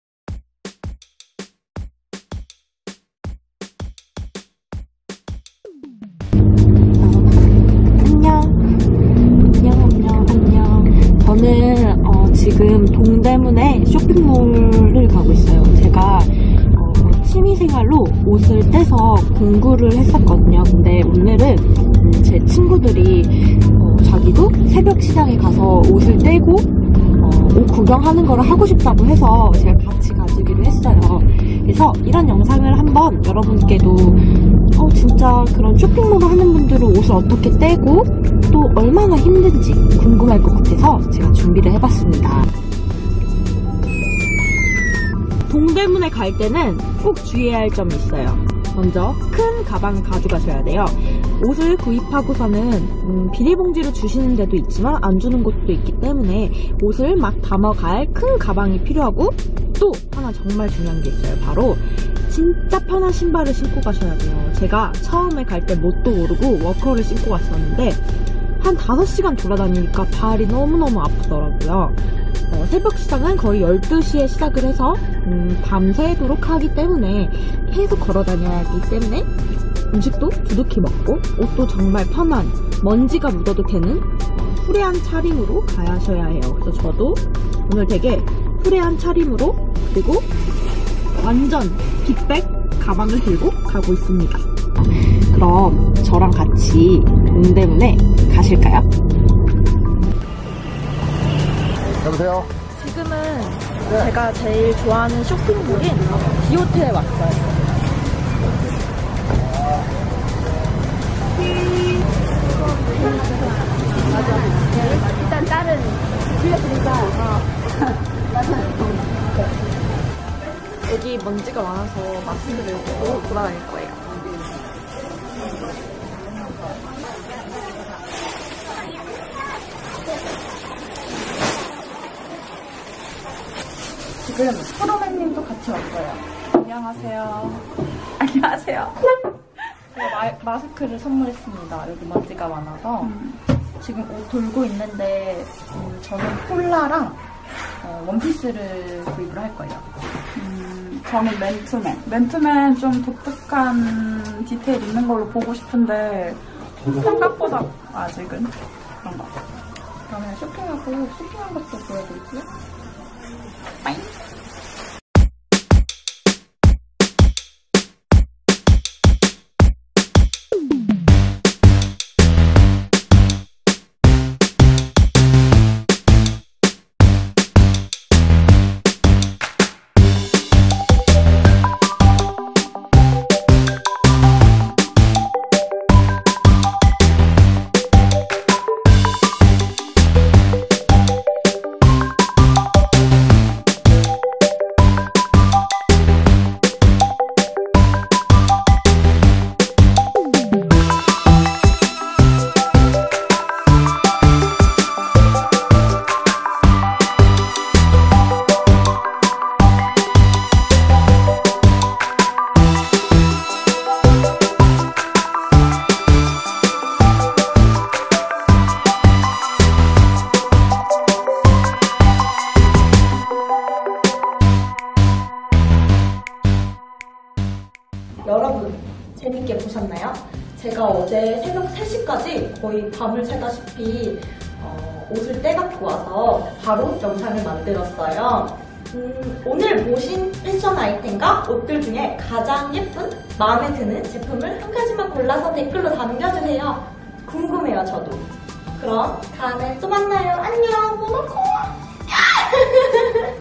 동대문 새벽시장 탈탈 털기 ! 옷 하울, 니트패션 데일리룩 민가든
안녕하세요. 민가든입니다.
저는 많은제품이 아닌 , 알짜베기 제품을 활용하는
튜토리얼을 만들고 있습니다.
데일리 메이크업부터 , 피부 나이 16살
꿀피부언니가 알려주는 스킨케어루틴을 볼 수 있어요.

오늘은 동대문 도매 새벽시장에서
쇼핑을 한 하울 ? 브이로그? 라고 해야할까용 ?

제가 취미로 옷을 이렇게 떼서
공구를 했었는데 ㅋㅋㅋ 사실..적자...ㅋㅋㅋ
택시비가 더 나왔더랬죠 !!!

도매와 소매 차이를 아셔야해요.
소매 = 우리들 . 도매 = 대량으로 옷 떼가는 (쇼핑몰)
도매로 구입을 해야해서 대량구매 . 깔별로 구매하셔야해요.

제가 쇼핑한 아이템
목걸이 , 퍼 팔찌, 니트 , 반바지 등등 !!
다 너무 너무 마음에 드네용 !!
헷

동대문새벽시장은 밤열두시에 열고 토요일 밤에는 운영을 안해용.